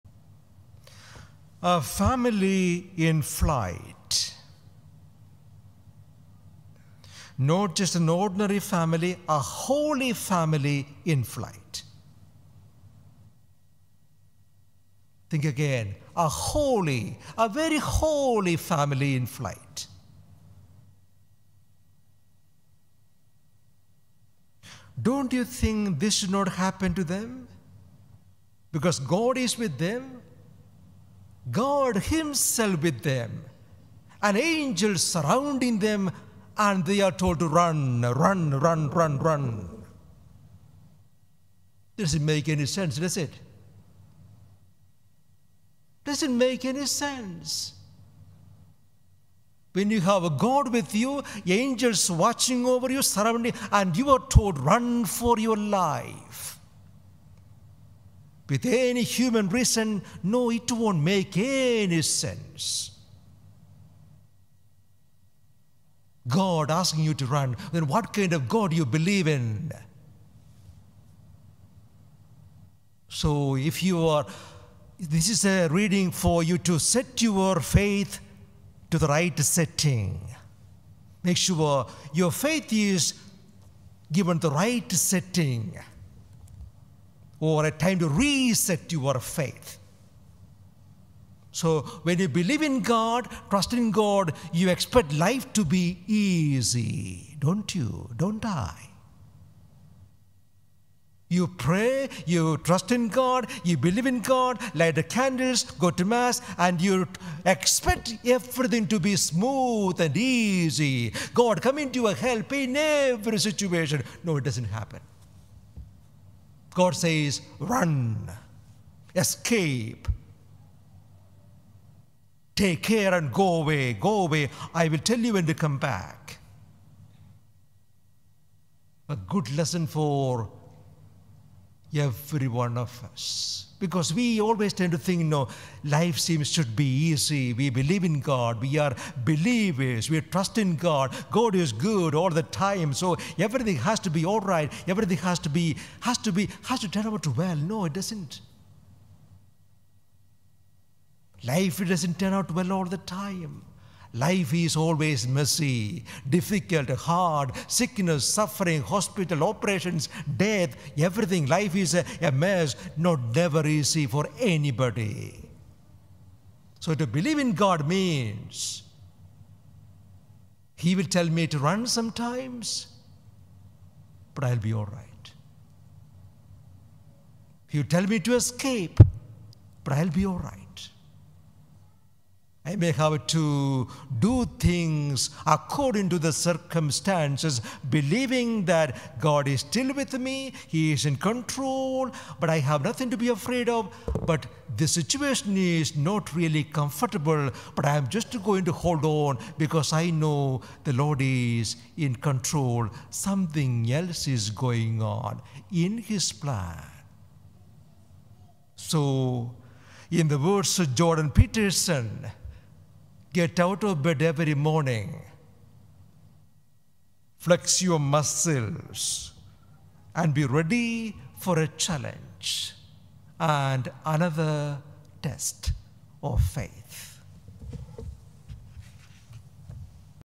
Homily 30Dec2022 Mt2:13-23 The Holy Family.That you trust in God doesn't mean your life will be easy
Get out of bed every day, flex your spiritual muscles and be ready for a challenge and a test of faith!